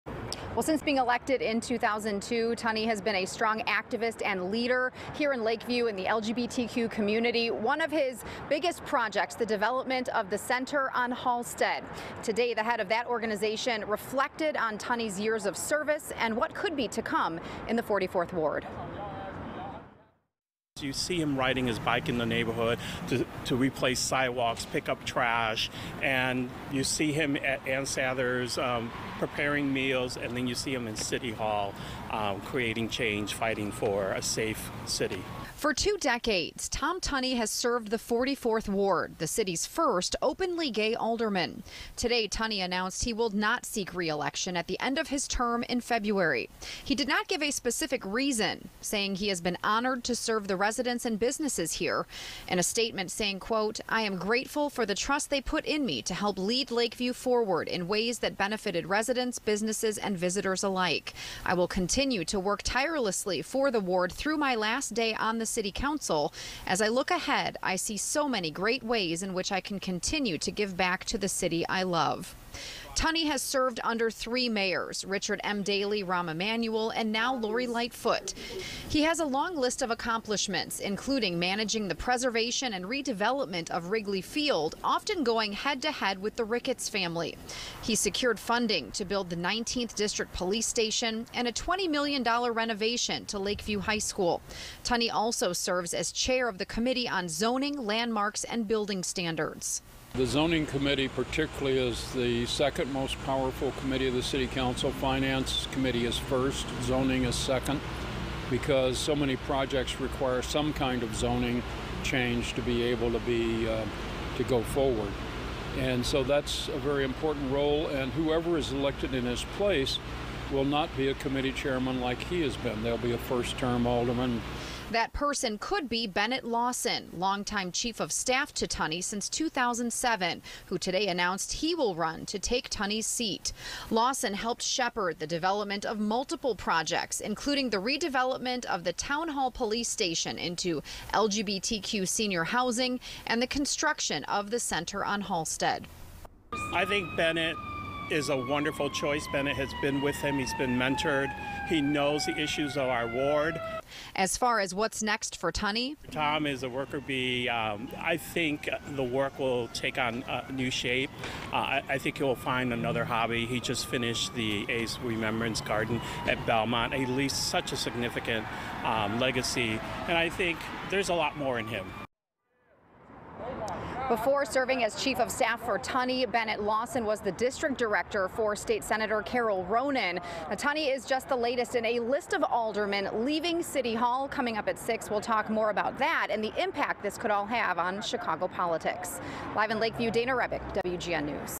Alderman Tom Tunney announces he will not seek re-election to city council
WGN's Dana Rebik dives into the career of Ald. Tom Tunney after he announces he will not seek re-election for his seat on city council.